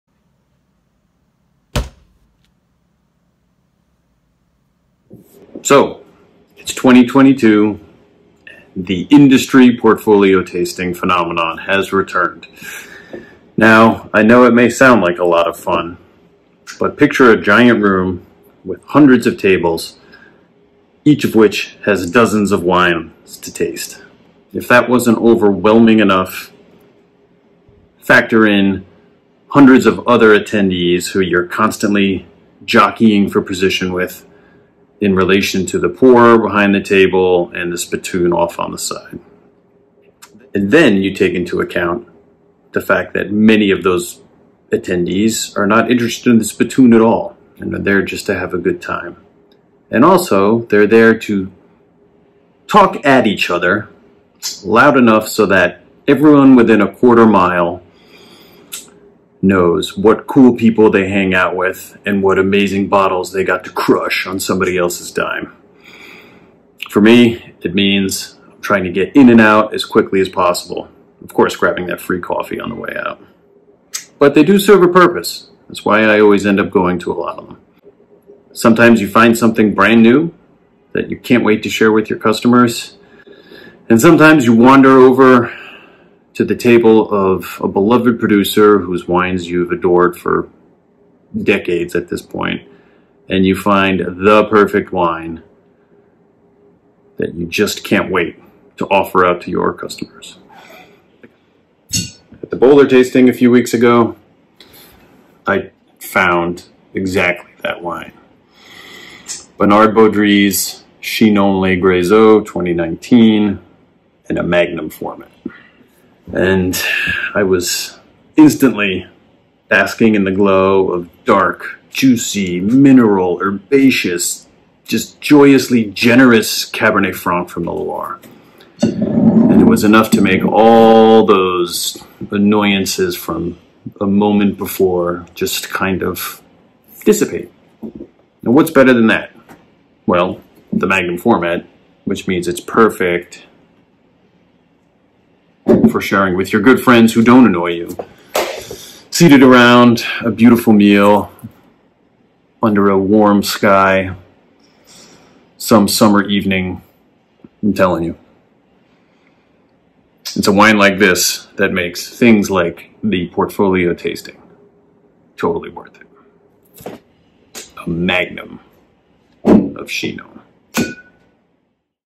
Baudry Chinon Grezeaux 2019 in MAG!!
Bernard Baudry Chinon Grezeaux 2019 in MAG!!
The phenomenon of the wine industry portfolio tasting has finally returned this year, aka my favorite thing on earth…
Sorry, I don’t mean to be so cynical and/or sarcastic, and I do still attend many of them because they do serve a purpose, and I know all of the planning and resources that each importer puts into making them happen, but the scene at them, for me, can be really hard to deal with.  A giant room, with an overwhelming number of tables to visit, each with a dozen wines on them is daunting enough as it is, but when you add in hundreds of attendees that you’re constantly jockeying for position with in relation to the pourer and the spittoon, and it makes focusing on each wine that much more difficult.  Then you remember that many of said attendees have no interest in spitting, or seem more interested in talking at each other just loud enough to let everyone within a quarter mile know how many cool people they’ve ‘crushed bottles’ with, or, even worse, are guilty of both bragging and not spitting, thus making the bragging get even louder and more irritating, and, well, for someone like me, you find yourself hyper-focused on tasting quickly, grabbing the free coffee and getting the eff out of there.
(And I’m usually there and gone early enough for the worst of that to have not even started…)
Let me be clear, though…if you’re the type who isn’t bothered by this, or has a good time in such a situation, I’m genuinely jealous of you.  I’d love to be able to hang out for hours on end and leisurely make my way through all of the wines in a deliberate way, but I’m not, and I’m usually rushing to get back to the shop anyway.
So, what’s the point of all of this poopie-pants-ing on my part?
The point is this - despite all of the distractions at big tastings, I almost always come across something that made it worthwhile.  Sometimes you discover something totally new, and you get really excited about getting on your shelf as soon as possible.  Or, as in today’s case, you decide to go taste the wines of a beloved producer whose work you’ve adored forever and you taste one of their current lineup that smacks the cynicism right out of your mouth and all of the noise around you seems to dissipate, and you find yourself steeped in a mix of both the pure joy that a fantastic wine delivers and the realization that said wine is exactly what your customers need heading into the upcoming season.
At the Bowler tasting a few weeks back, that table was Bernard Baudry, and that wine was the 2019 Chinon Grezeaux in a magnum format.  Put simply, it is both classic Chinon and classic Baudry, presented in a holy-crap-that's-drinking-now-but-I-also-can't-wait-to-see-it-in-5-to-7-years package.  From the wonderfully generous 2019 vintage, it’s dark, juicy, herbaceous, firmly mineral and beautifully aromatic and…well…just yum…
I mean, it’s everything you want, especially for an al fresco dinner some evening this Summer.The magnum format makes it all the more fun and perfect for sharing.  And for those of you Baudry purists, yes, it’s going to age beautifully too…most especially in the greatest-for-aging vessel known as the magnum.
Perfect Chinon, perfect volume, and perfect at making the noise and distractions of life melt away and the beauty of all things come sharply into focus, where they belong.

A Magnum of Chinon…

…just perfect…

Matt Franco